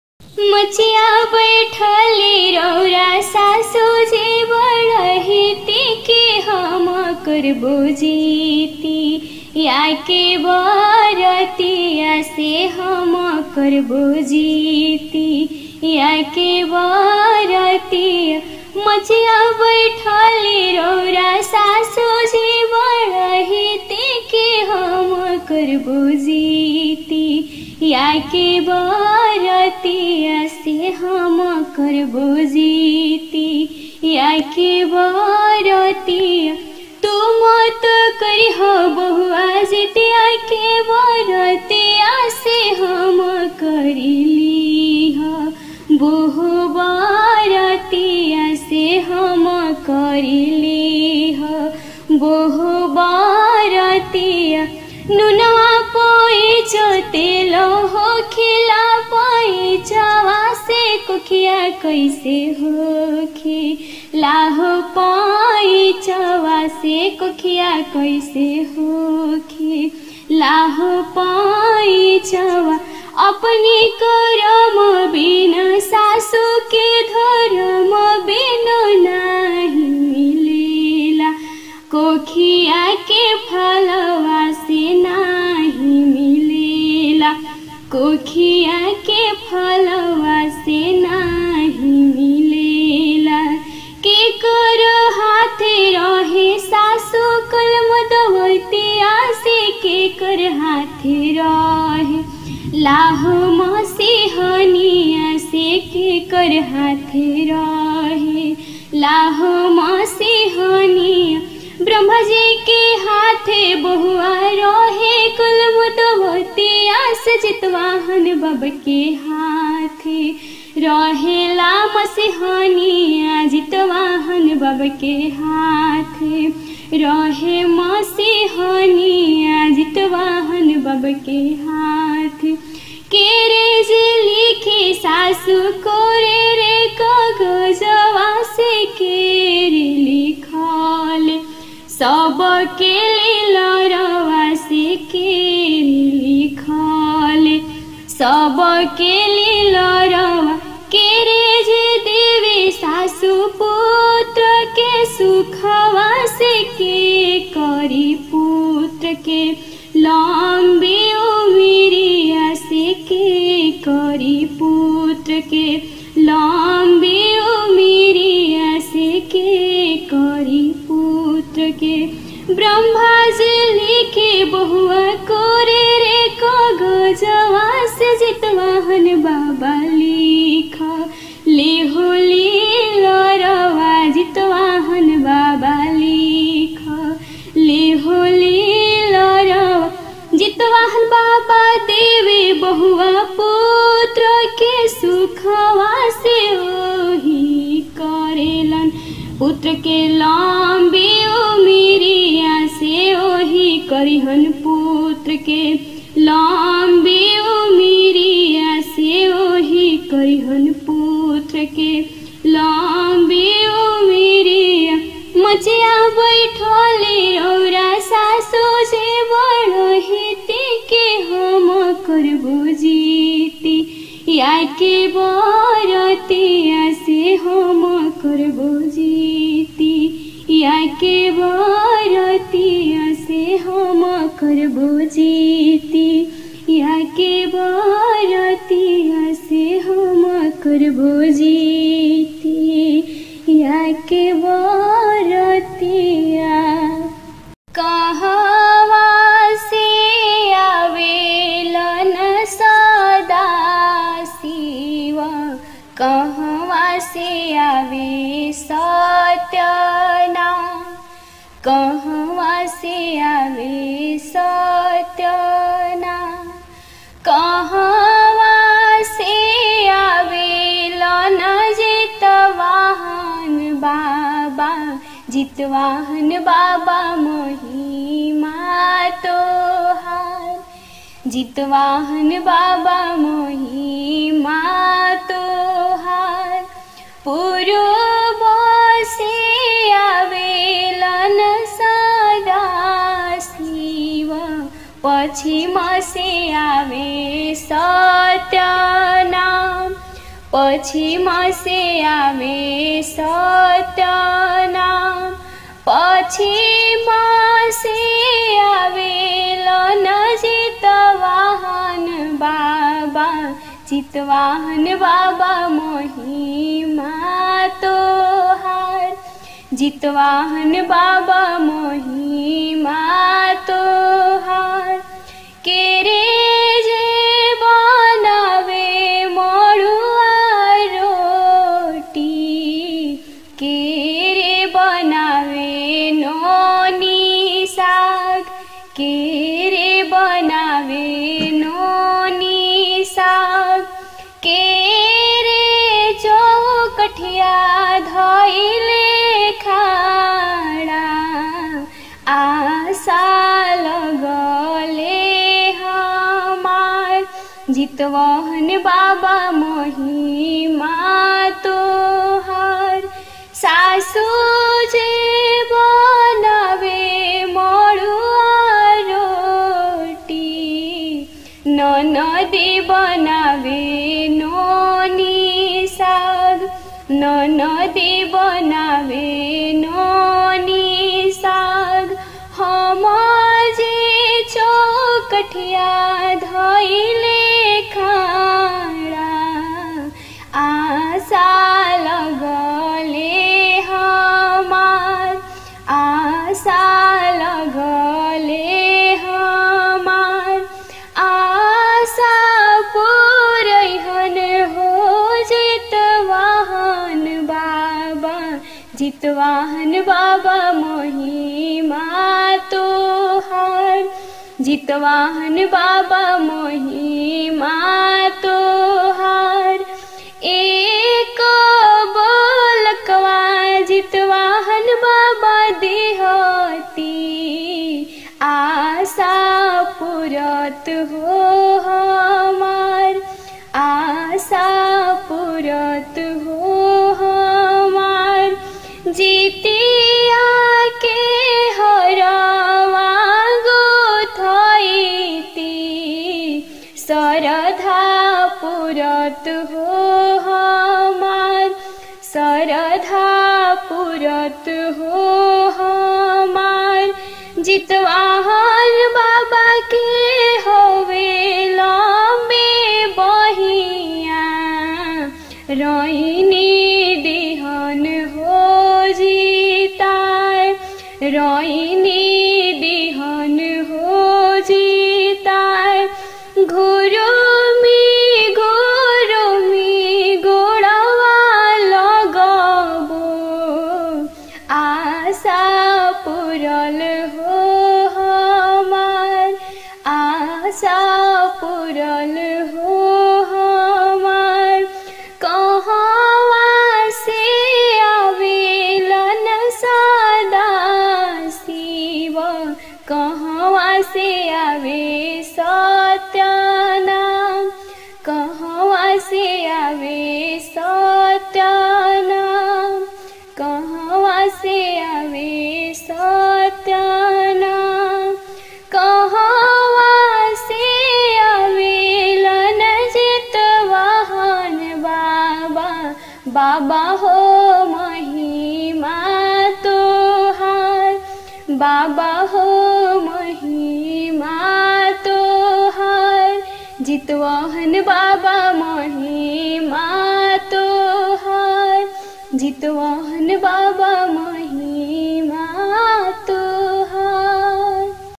घर घर में बजने वाला पारम्परिक जितिया गीत || Anshu Priya Bhojpuri Bhakti Song 2020
Song - Jitiya Geet
Singer - Anshu Priya
Category - Bhojpuri